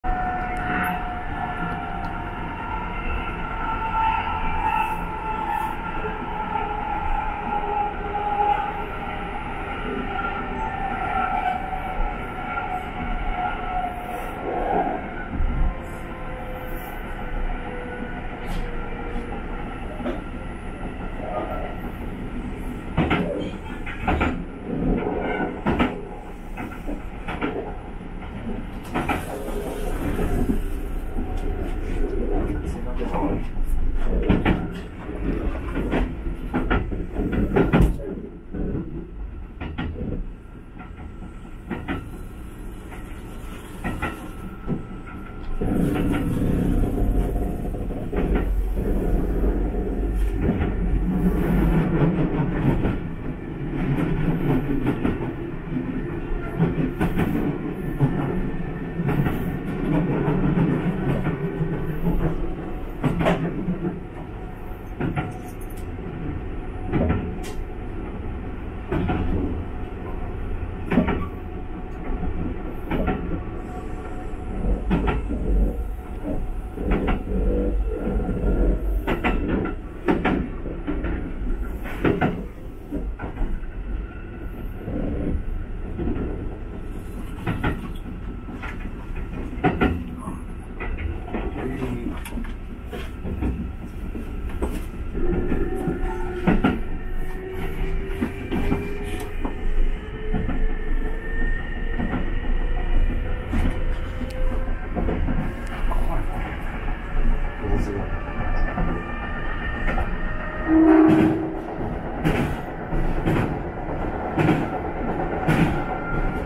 近鉄 中川短絡線の前面展望 Triangular junction, Kintetsu Ise-Nakagawa Station (2020.8)
近鉄伊勢中川駅構内の大阪線から名古屋線への短絡線、中川短絡線通過のアーバンライナーからの前面展望です。
2020年8月、DJI Osmo Pocketで撮影。

Mie Japan railways trains
近畿日本鉄道　三重県松阪市　電車